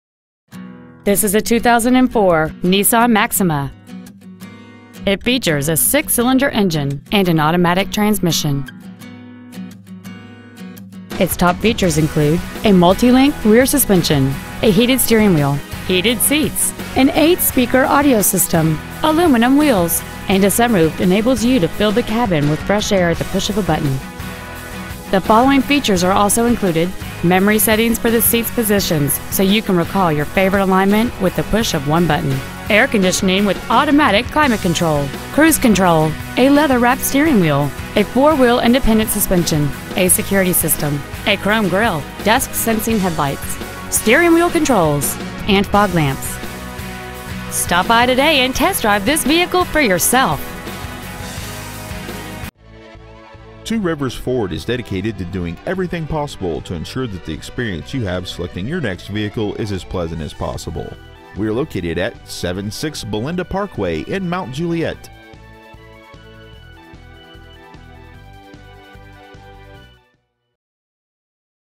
2004 Maxima Mount Juliet TN
We are proud to present this 2004       Maxima .

 We have been honored to serve the Mount Juliet TN area , we promise that your experience at our dealership will exceed your expectations !

 Year : 2004
 Make :
 Model : Maxima
 Engine : 3.5L V6 24V MPFI DOHC
 Trans . : AUTOMATIC
 Exterior : GOLD
 Miles : 94,213
 Interior : BEIGE
 Stock : 40054-1